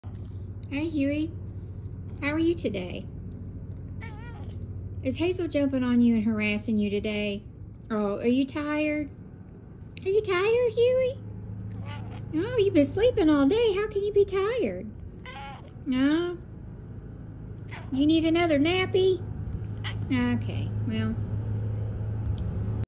What Happened When I Started Talking to My Cat?
In this video, I talk to my cat about his day and he responds to me. Watch as he responds with cute, strange, and hilarious expressions!

Talking to your cat is a great way to connect with them and learn about their life. In this video, we'll be taking a look at what happened when we started talking to our cat and how it worked out. From hilarious expressions to cute cat interactions, this video is sure to make your day!